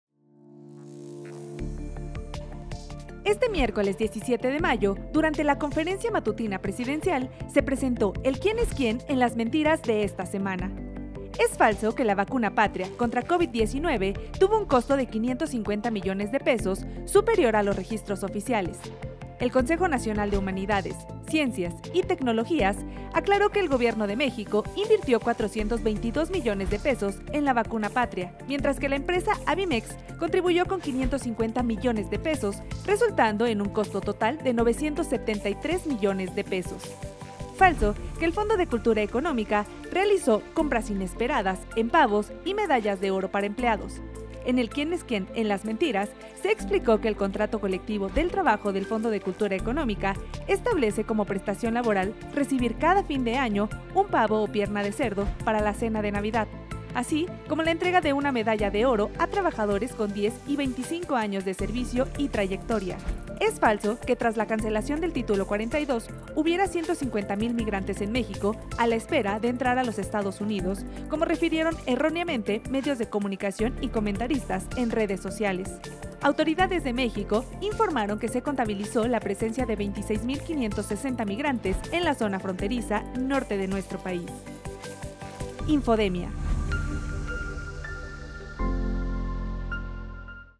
El Quién es Quién en las mentiras del 17 de mayo de 2023
El QEQMentiras del 17 de mayo

❌Falso que la vacuna Patria contra Covid19 tuvo un costo de 550 mdp superior a los registros oficiales
❌Falso que el FCEMexico realizó “compras inesperadas” en pavos y medallas de oro para empleado